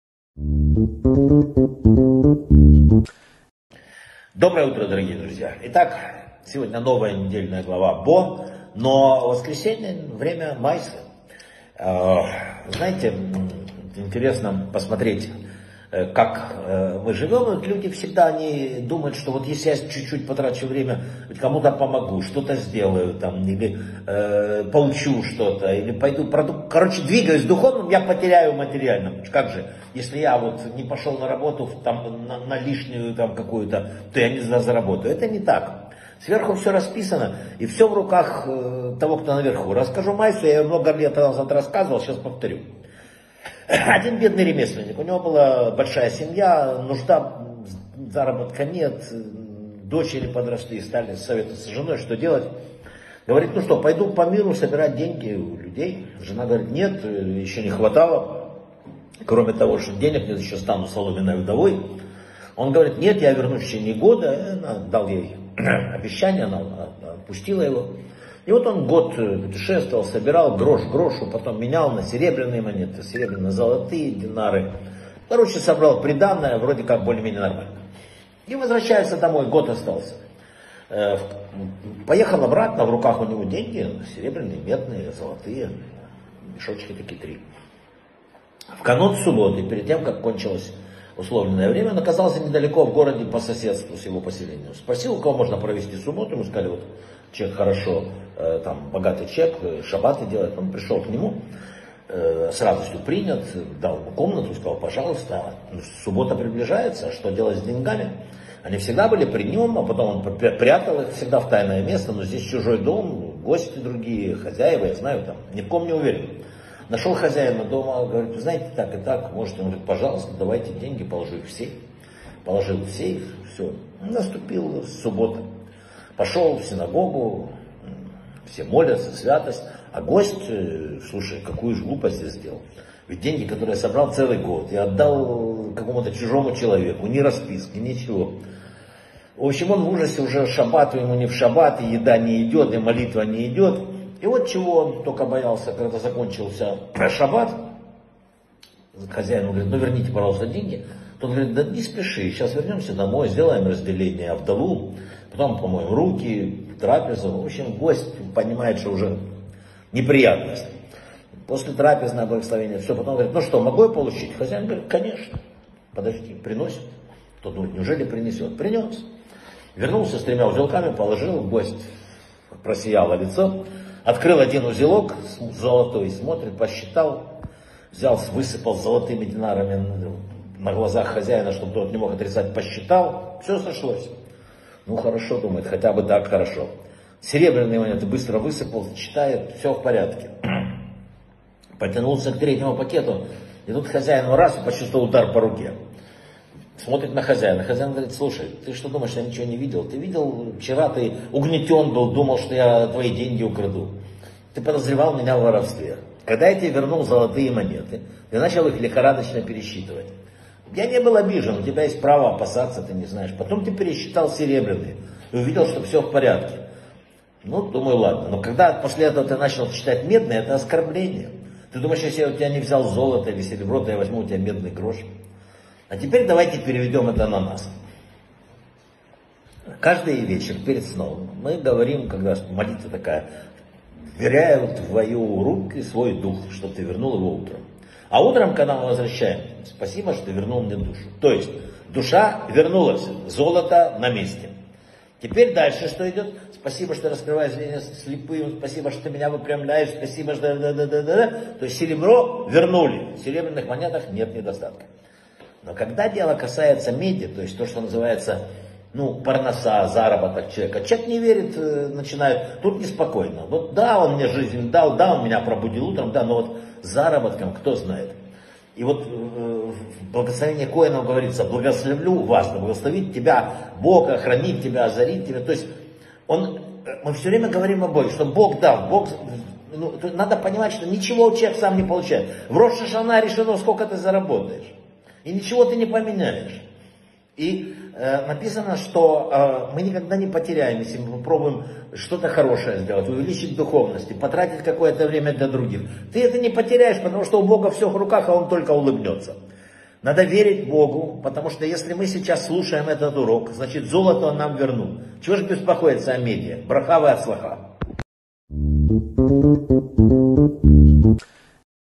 📘 Заработок человека - без сомнения в руках Творца | Вадим Рабинович
Почему ошибается человек, полагающий, что уделяя больше времени духовному, он потеряет в финансах и материальном благополучии?
История о еврее, который год работал и копил деньги на приданое для дочери. Что с ним произошло перед Шабатом, накануне возвращения домой?
Как отреагировал хозяин дома, где остановился этот человек на Шабат, когда тот начал пересчитывать свои монеты?
Что мы говорим в молитве каждый вечер перед сном и как это отражает актуальность этой истории для каждого из нас? Что сказано в благословении коэнов? Помним о том, в чем источник всего.